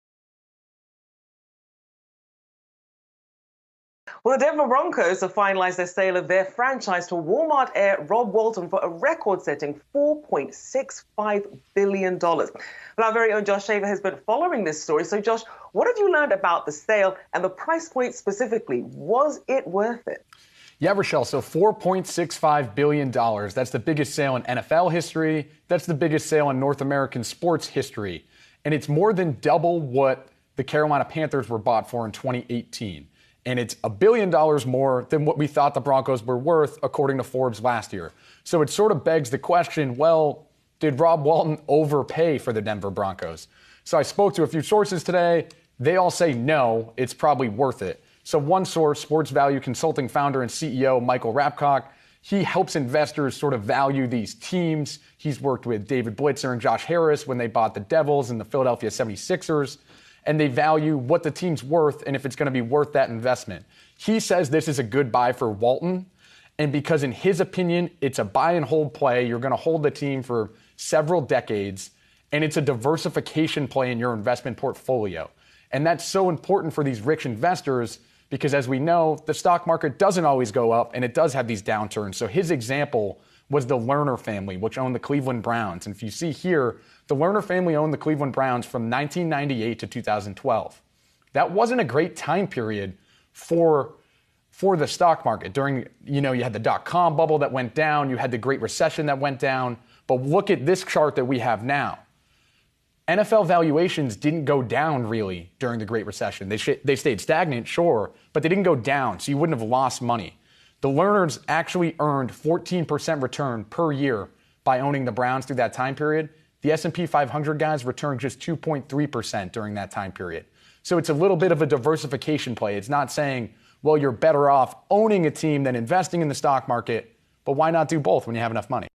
Walmart heir Rob Walton officially buys Denver Broncos football team for $4.65 billion
Yahoo Finance reporter Josh Schafer breaks down the $4.65 billion purchase Rob Walton made for the Denver Broncos.